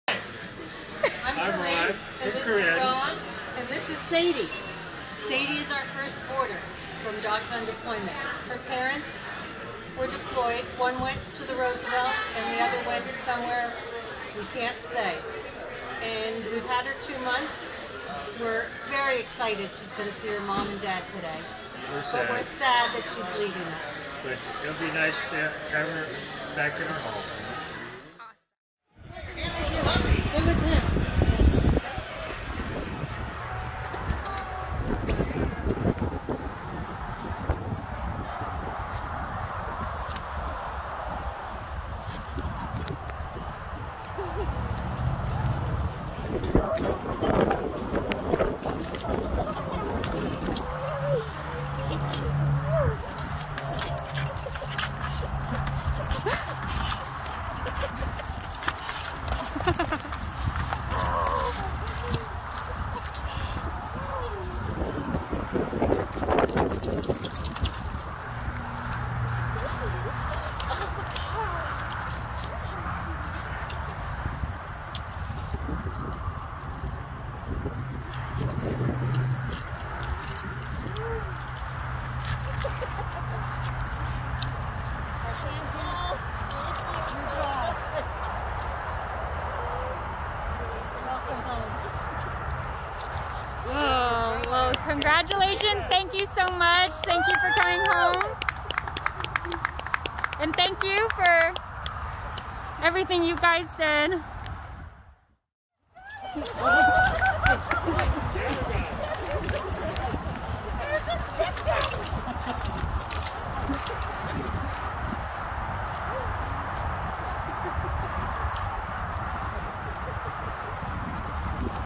Dog Reunites With Military Owner In Time For Holidays
An excitable Labrador could not hide her delight, as her military mom returned from deployment just in time for the holiday season. Joy Nameth is a lieutenant in the U.S. Navy, and in the past she has been deployed to Bahrain twice – once for six months and another for eight months. Such travel can be tough on Joy’s pooch, Sadie, 9, so for her most-recent deployment, Joy reached out to Dogs on Deployment, which specializes in finding temporary homes for military members' pets. For her most-recent deployment, Joy, who lives in Colorado, was swiftly called to serve on the USS Makin Island, which saw her leave home for around two months. And within 24 hours of posting a listing through Dogs on Deployment, Sadie had already found a temporary home. When the lieutenant returned, on November 8, Dogs on Deployment also helped plan a reunion with Sadie, in Oceanside, California. With Joy waiting outside, Sadie was walked toward her owner on her leash. As soon as she caught sight of Joy, Sadie immediately recognized her.

VideoID: TT-4817